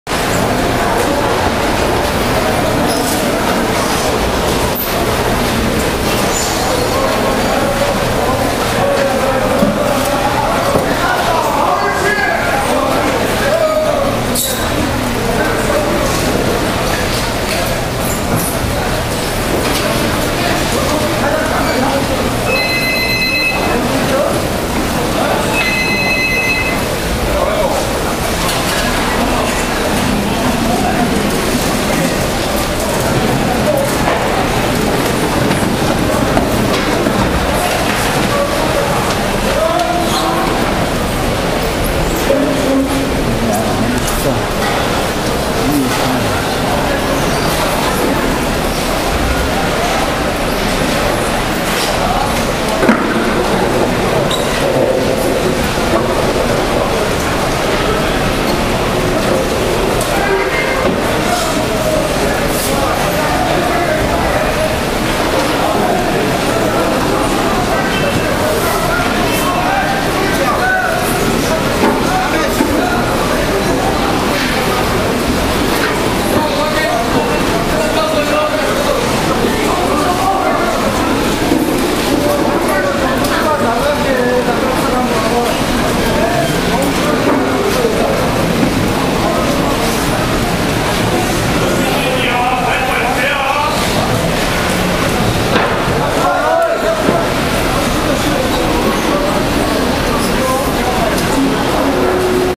노량진 수산시장 새벽(1) 어물전 빛내는 꼴뚜기
11월29일 오전 5시20분경에 찾아간 노량진 수산시장의 환한 불빛. 어획량 감소로 귀하신 몸 대접 받는 꼴뚜기를 포착했다.